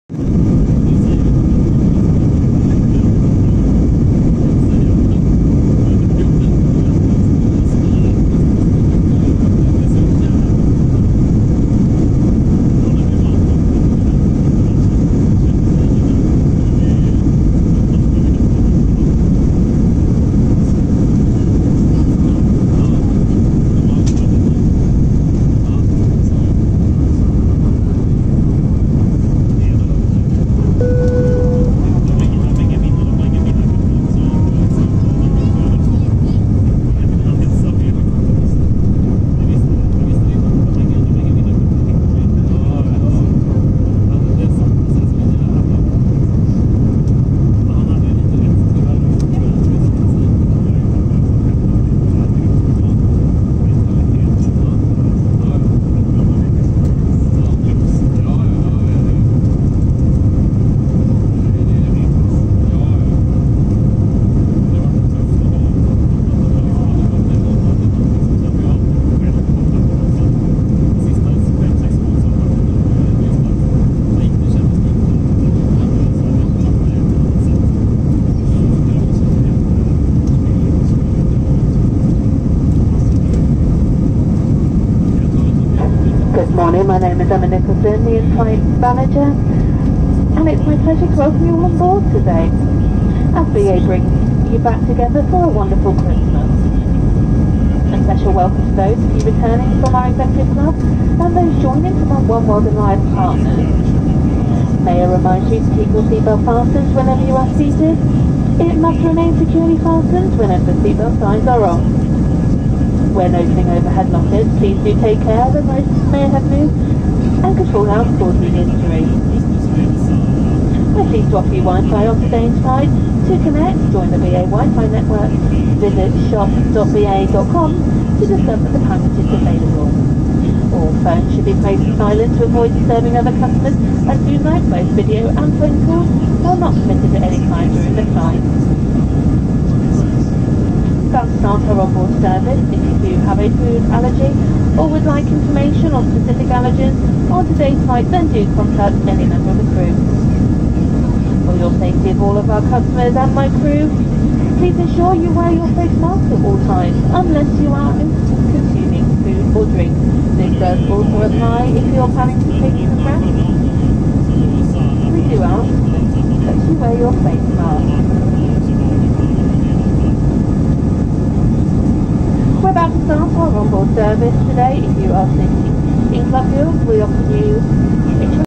Göteborg Landvetter Take-off In The Snow
Probably the heaviest snow I've seen in Göteborg in the 5 years I've been here. Travelling to the UK for the first time in 2 years (since Covid) to see family. This BA flight had to be de-iced before take-off but it made it into LHR on time.